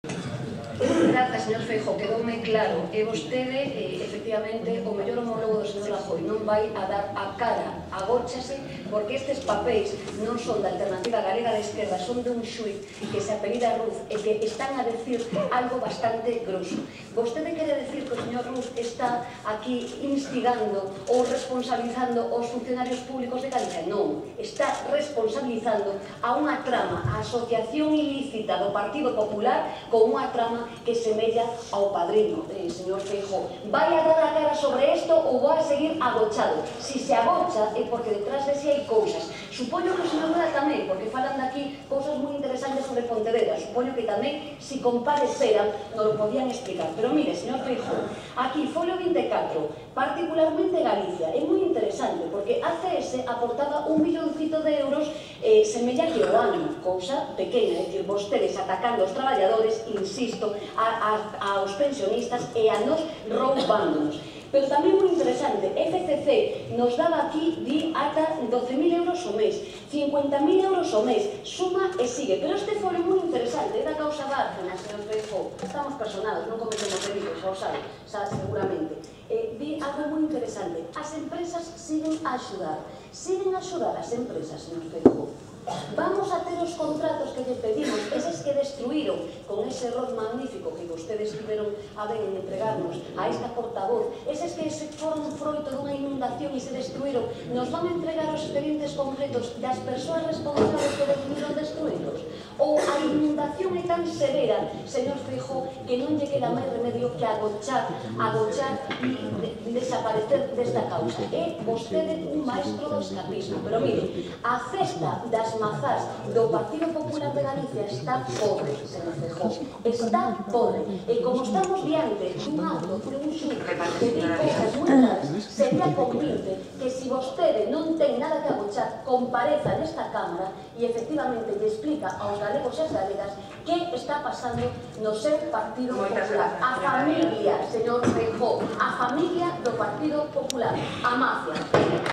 Yolanda Díaz acusa a Feijóo de pertencer 'A famiglia do PP, a mafia'
A viceportavoz de AGE, Yolanda Díazm insiste en esixir unha explicación ante a falla de resposta do presidente da Xunta, Alberto Núñez Feijoo, sobre o suposto financiamento ilegal do seu partido a nivel estatal e tamén en Galicia.,